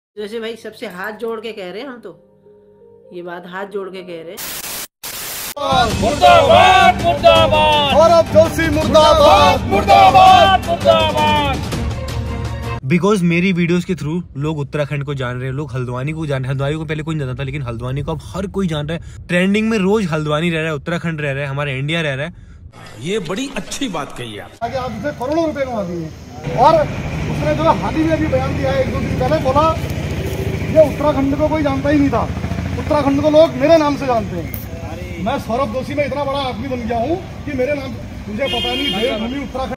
Big News ! Sourav Joshi Ne Kya Bol Diya | Sourav Joshi Ka Reply |Sourav Joshi Ka पुतला जलाया Diya 😲
Original Credits:-

souravjoshivlogs song